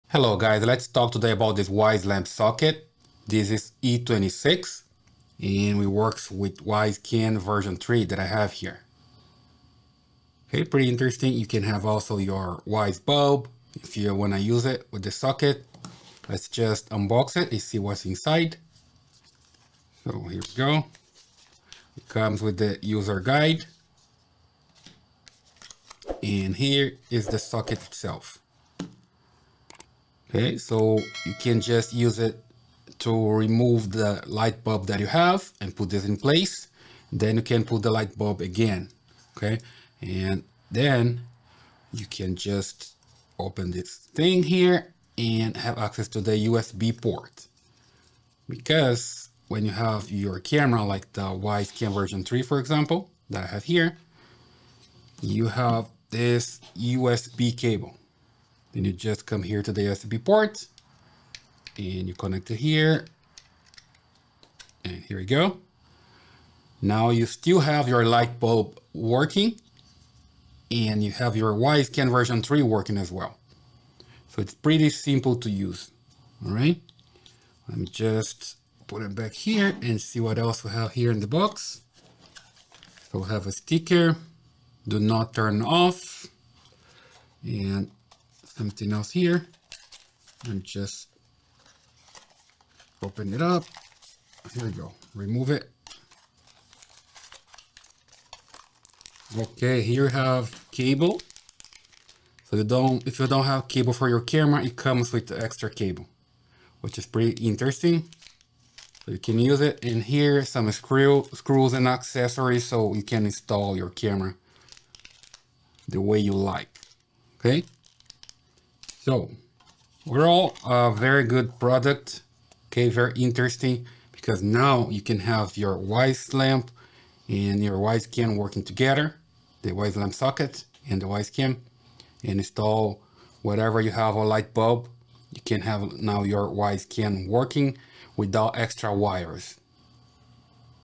Wyze Lamp Socket Review
Wyze Lamp Socket for Wyze Cam V3

The Wyze Lamp Socket Power Adapter for Wyze Cam v3 is a must-have accessory for those who want to expand their home security system. This product allows users to easily power their Wyze Cam v3 directly from any standard lamp socket.

Made with high-quality materials, the Wyze Lamp Socket Power Adapter is durable and long-lasting. It is also easy to install and does not require any complicated wiring or installation processes. Simply screw in the adapter into the lamp socket and attach the Wyze Cam v3 to the adapter.

With the Wyze Lamp Socket Power Adapter, users can easily mount their Wyze Cam v3 to any location they desire without the need for a nearby power outlet. This provides users with greater flexibility and convenience when setting up their home security system.

Overall, the Wyze Lamp Socket Power Adapter for Wyze Cam v3 is a reliable and efficient accessory that is sure to enhance the functionality of any Wyze Cam v3. It is a must-have for anyone looking to expand their home security system or increase the convenience of their current setup.